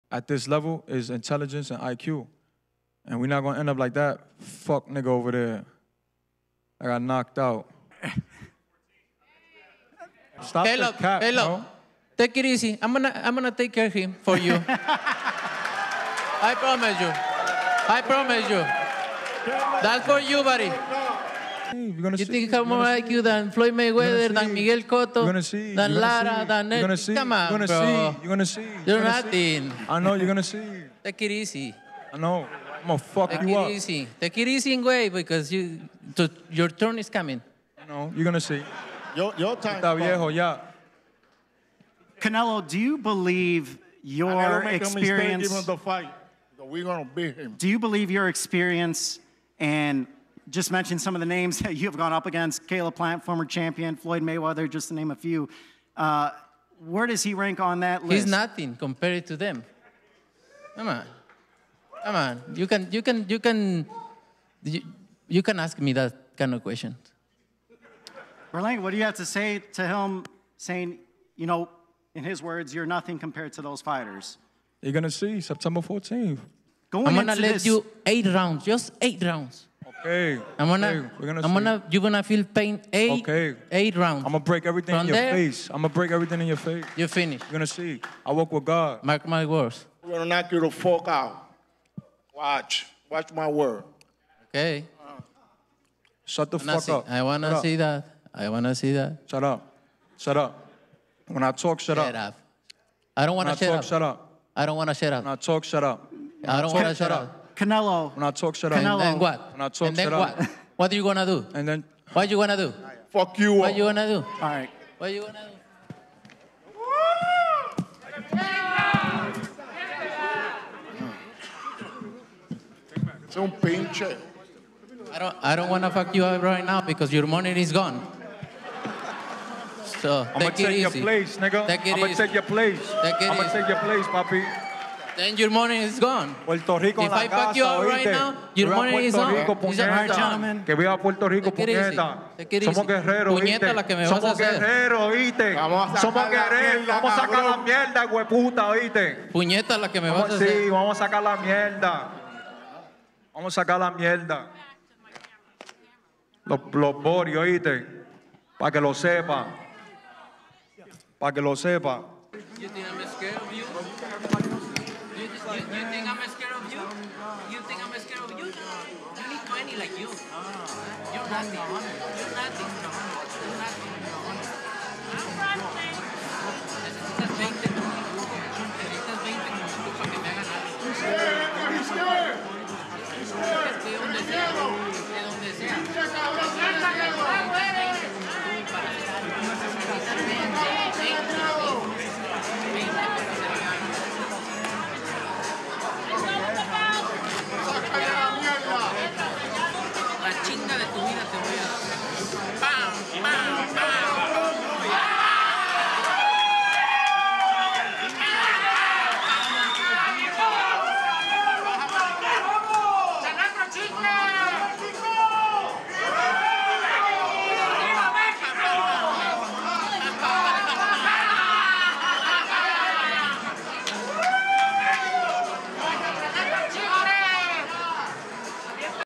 Canelo Alvarez and Edgar Berlanga exchange heated words in L.A.
Saul "Canelo" Alvarez and Edgar Berlanga Exchange Heated Words in their Los Angeles press conference on August 6, 2024.